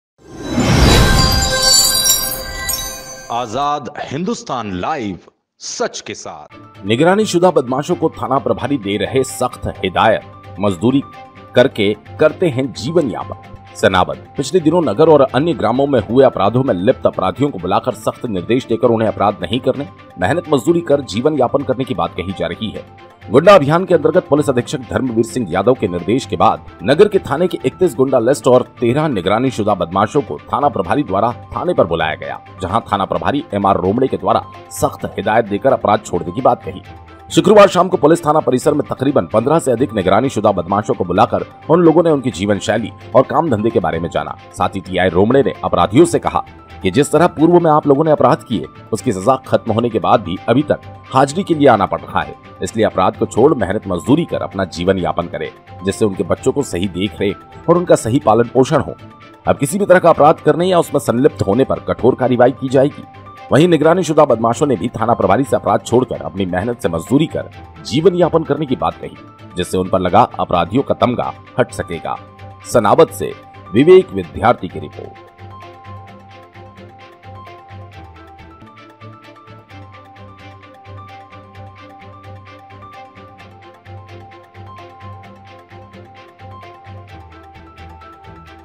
निगरानीसुदा बदमाशों को थाना प्रभारी दे रहे सख्त हिदायत
मजदूरी कर के करते है जीवन यापन